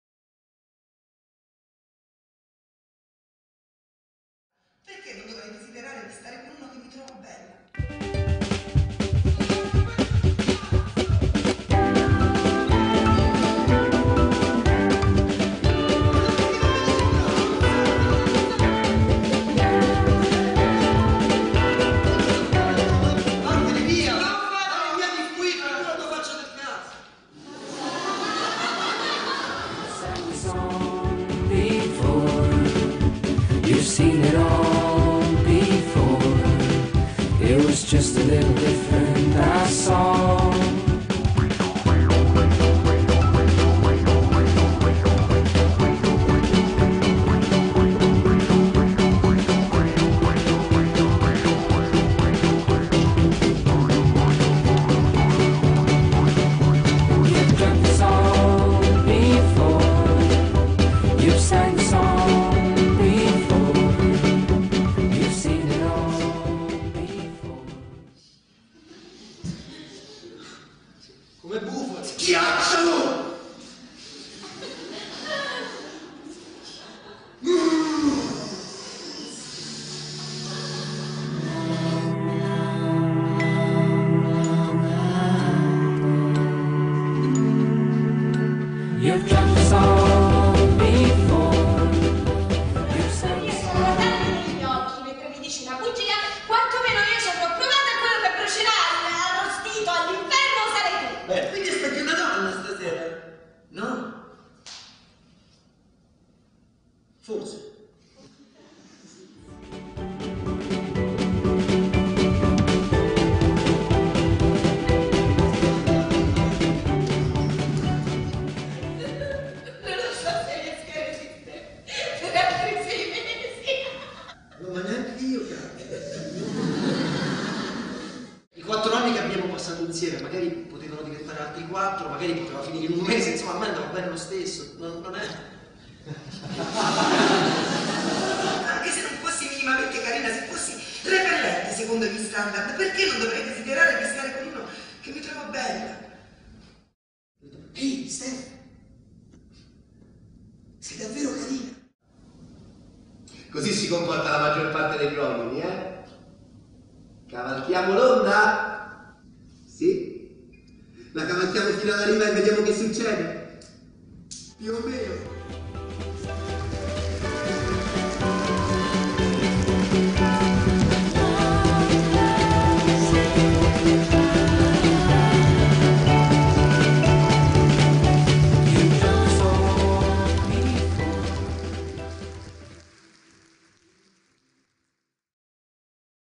Pretty Un motivo per essere carini
di Neil LaBute traduzione di Lorenzo Amato
con Filippo Nigro, Fabrizia Sacchi, Giulio Forges Davanzati, Dajana Roncione
regia Fabrizio Arcuri